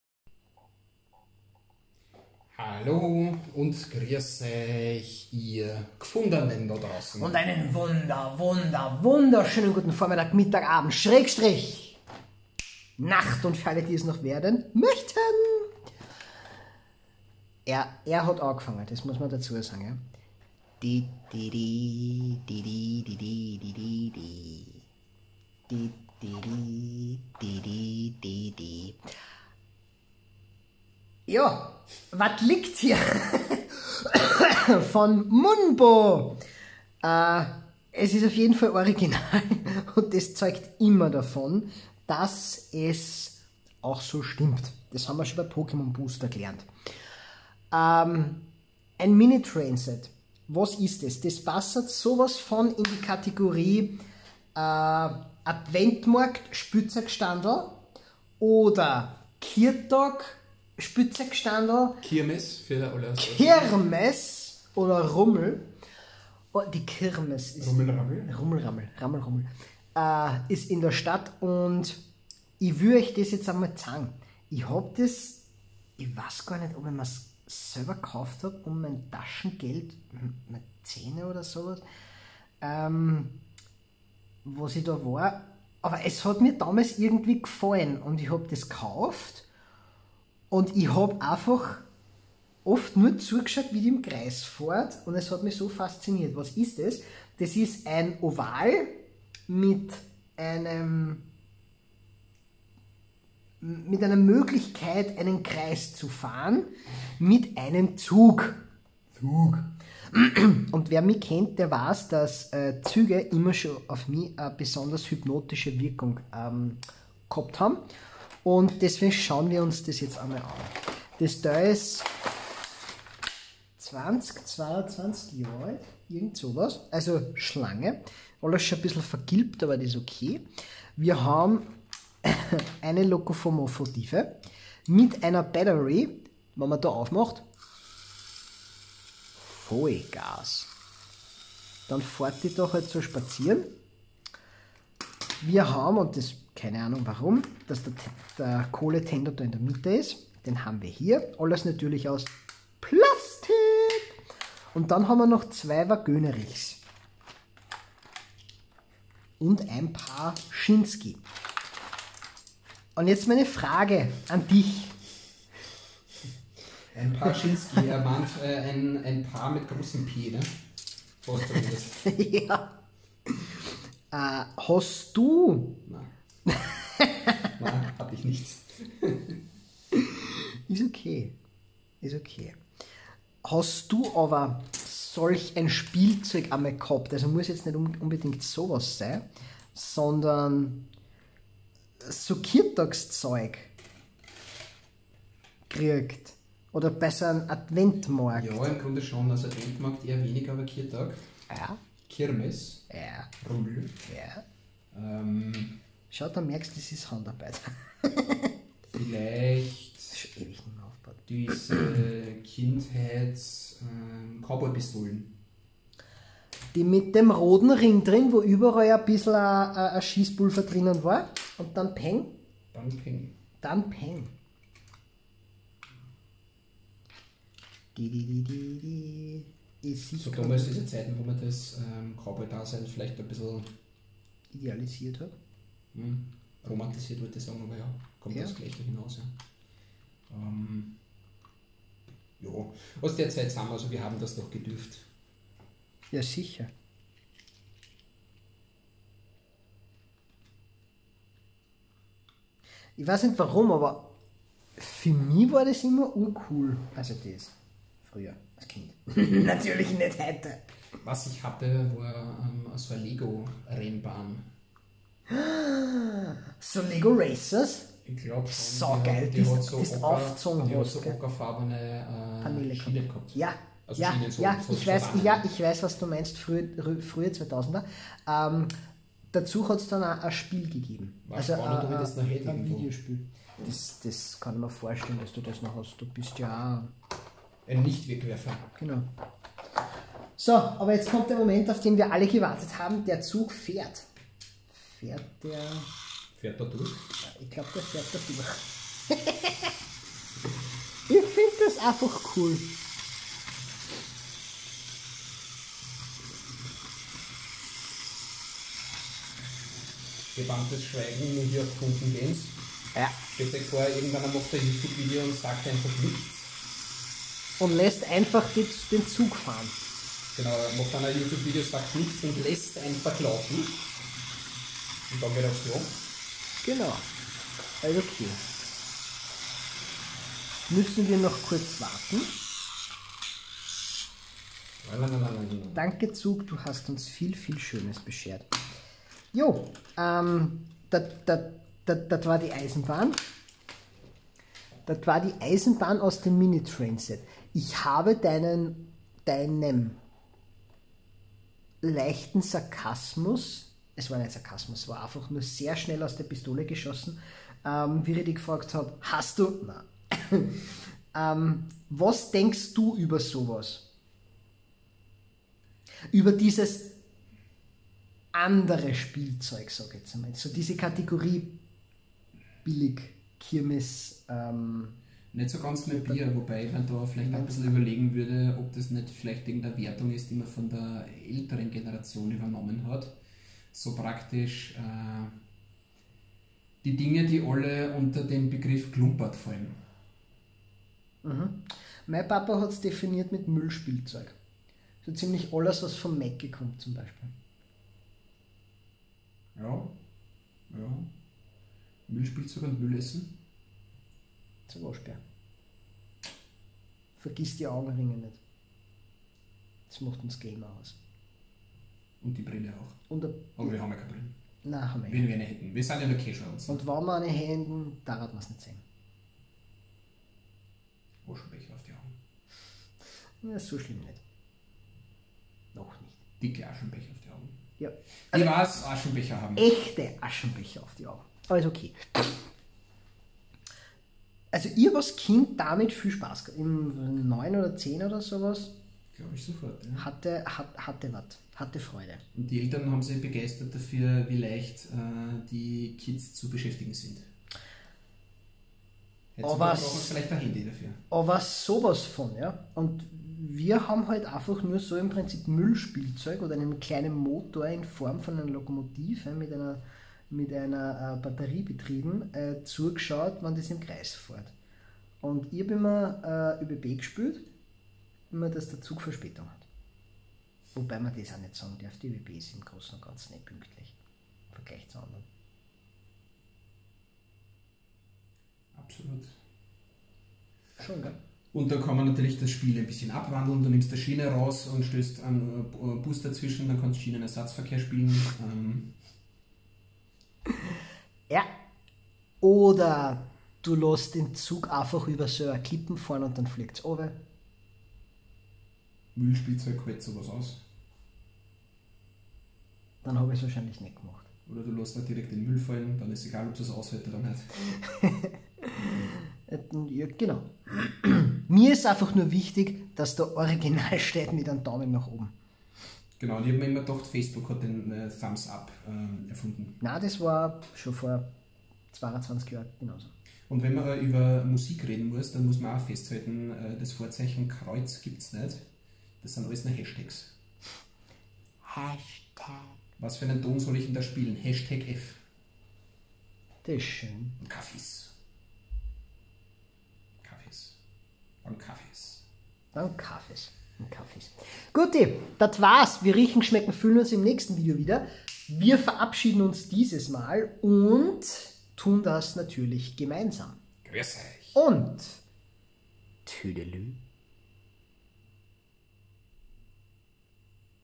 Mini Train Set vom Adventmarkt 2002
und heute zeigen wir euch eine Kindheitserinnerung vom lieben Rok. Neuerdings wieder gefunden und gedacht, dies möchte ich euch zeigen! Welch großartige Möglichkeit es zu präsentieren. Und Modellbau und Züge haben mich schon immer interessiert ;)
viel spaß und schönen Tag noch!
dok und rok!